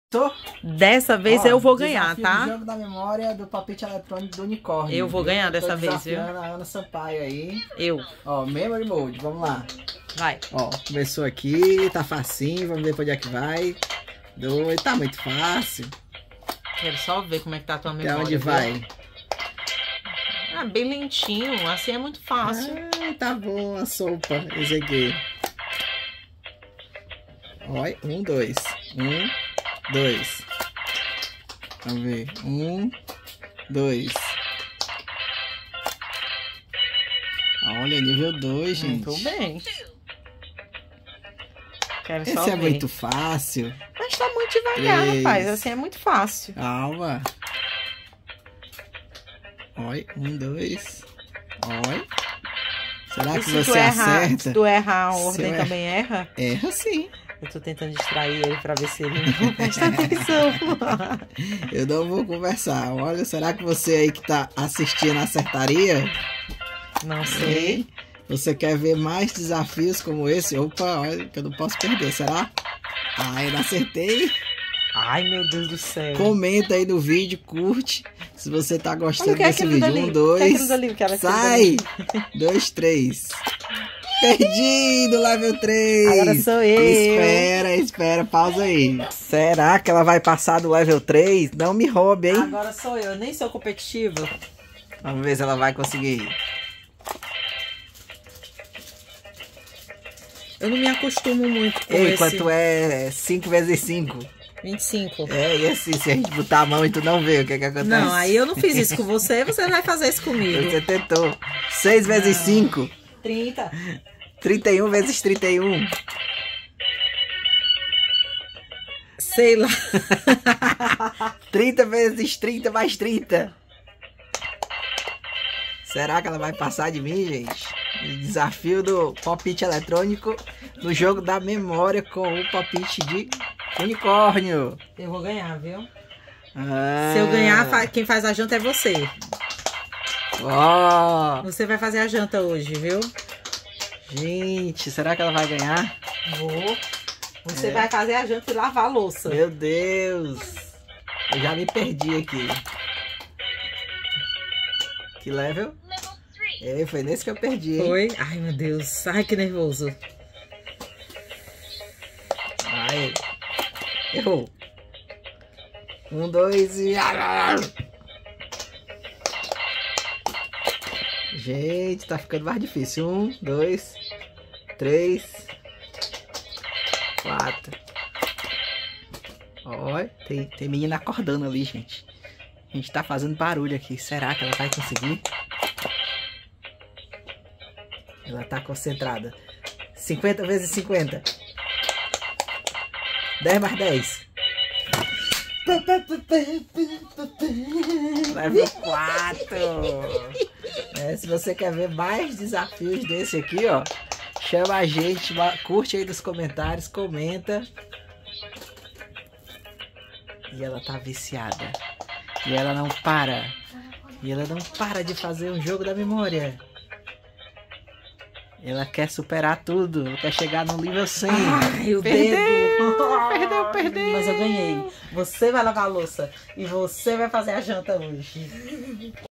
Desafio Pop it Eletronico Unicórnio Jogo da Memória - Push Game Brinquedo Para Criancas
"Pop it" é um brinquedo de mão simples que ganhou popularidade em 2021. Consiste em uma placa de silicone com bolhas que podem ser estouradas pressionando-as para baixo. É frequentemente usado como um redutor de estresse ou brinquedo para inquietação. A versão eletrônica pode incluir recursos adicionais, como efeitos sonoros ou luzes, aprimorando a experiência sensorial de estourar as bolhas. Essas versões eletrônicas podem ser encontradas em várias formas, tamanhos e designs, proporcionando entretenimento tanto para crianças quanto para adultos.